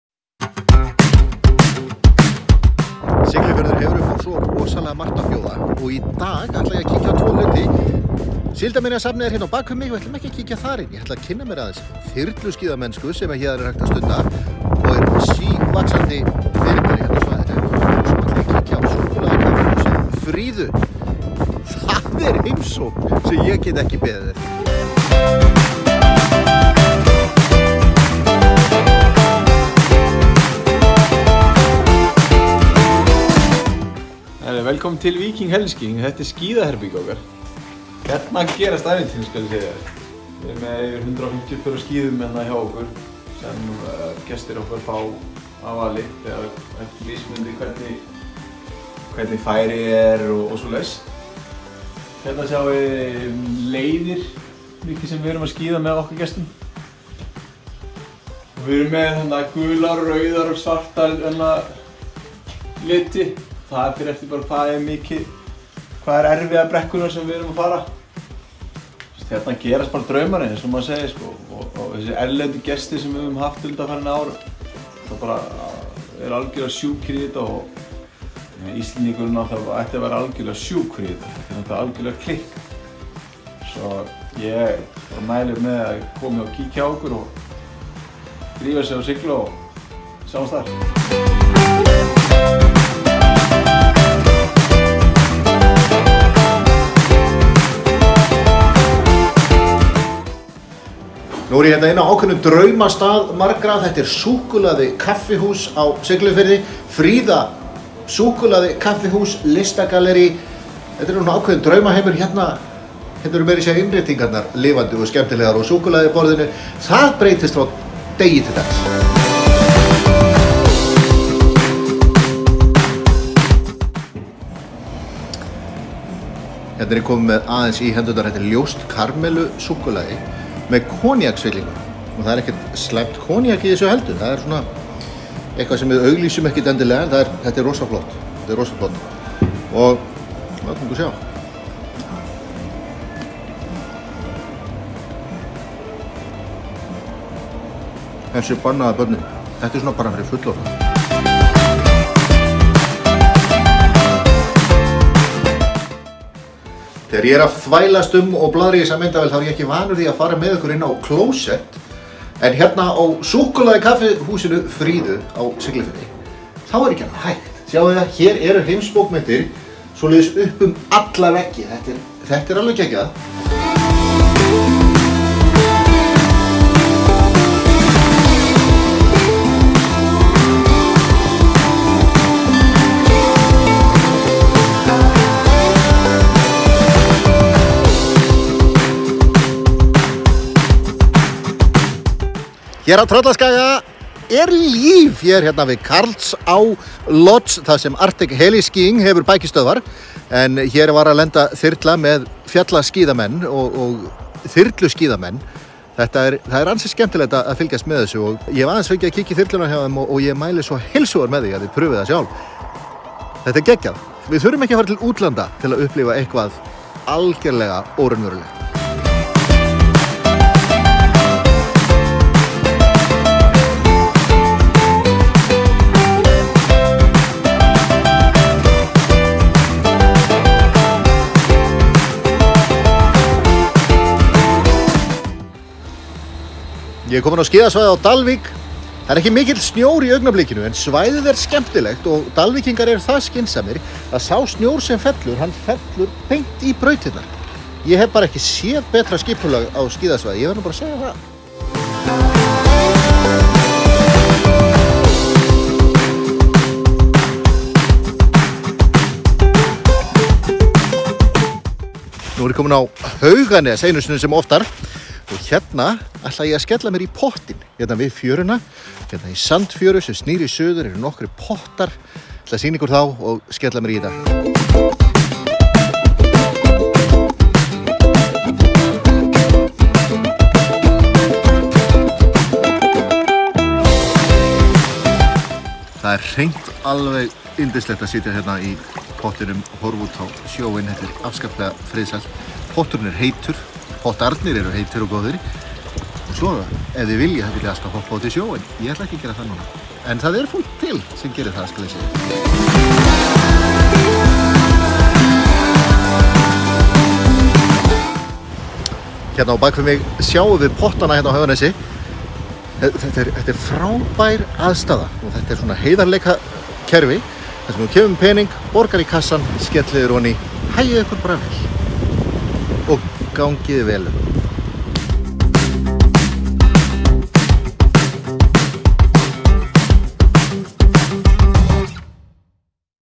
Chocolate café, heliskiing and hot tubs by the beach
Júlli the Tour Guide is on the Troll peninsula where he visits heli-skiing companies, before heading to a café that sells hand-made chocolate. He also travels to Hauganes to take a bath in the hot tubs by the black sand beach.